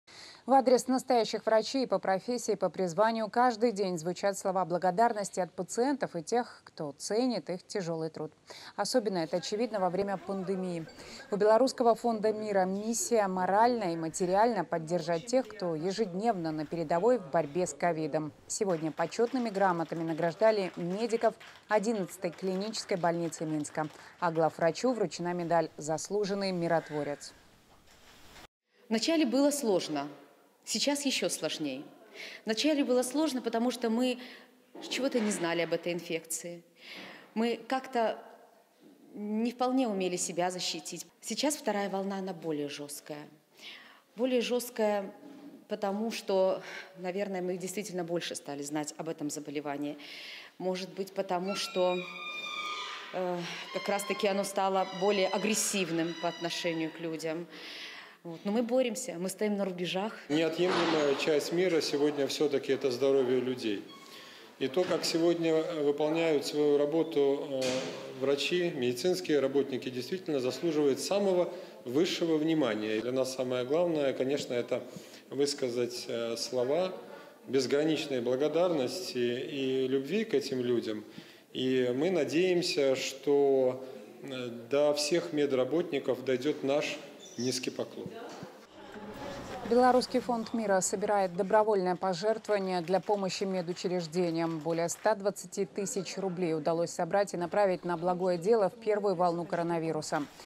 "Поклон Вам Низкий – Наши Доктора": Специальный репортаж ОНТ
Этот видео-репортаж от ОНТ является глубоким и искренним признанием в благодарности нашим медицинским работникам. "Поклон Вам Низкий – Наши Доктора" - это не просто слова, это выражение всенародной признательности и уважения к тем, кто посвящает свою жизнь заботе о здоровье и благополучии общества. В этом репортаже мы знакомимся с историями врачей, их ежедневным героизмом, самоотверженностью и профессионализмом. Это видео - дань уважения всем медицинским работникам, которые каждый день стоят на передовой здравоохранения.